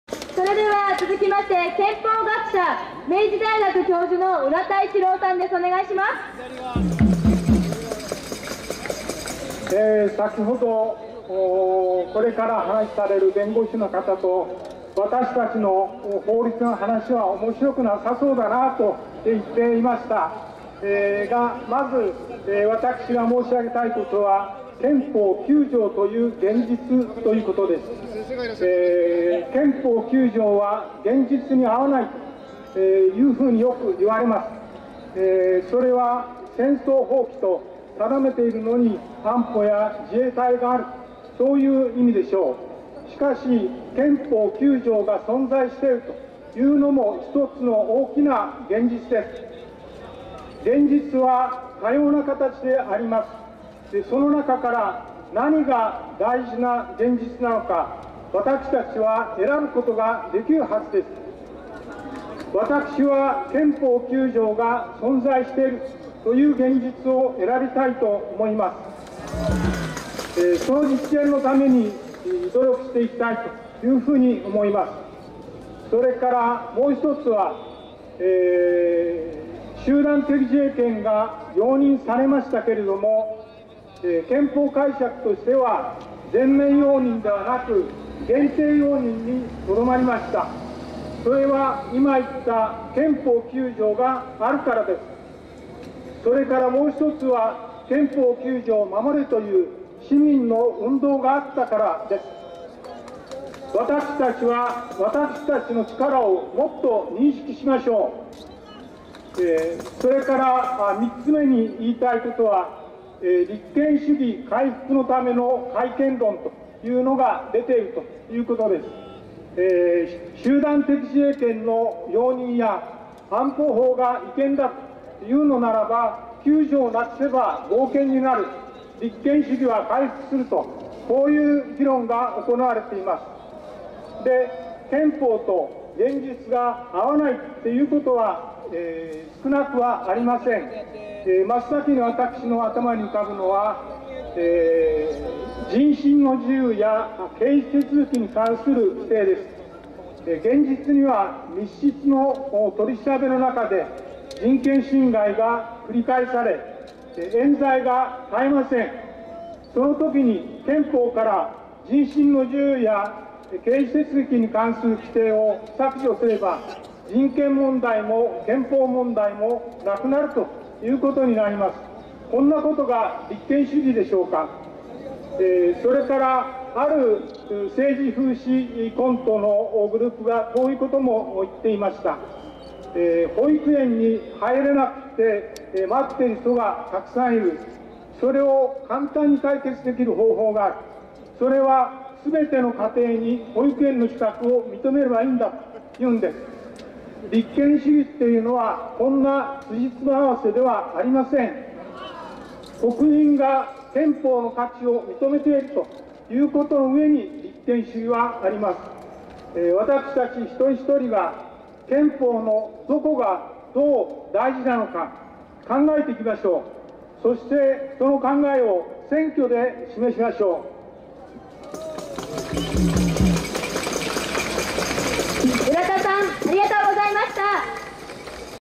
浦田一郎氏 スピーチ［明日を決めるのは私たち － 政治を変えよう！6.5全国総がかり大行動］ 2016.6.5
2015/06/05 国会周辺で行われた、戦争させない・9条壊すな！総がかり行動実行委員会／安保法制の廃止と立憲主義の回復を求める市民連合の主催による集会、『明日を決めるのは私たち － 政治を変えよう！6.5全国総がかり大行動』での憲法学者で明治大学教授の浦田一郎氏のスピーチです。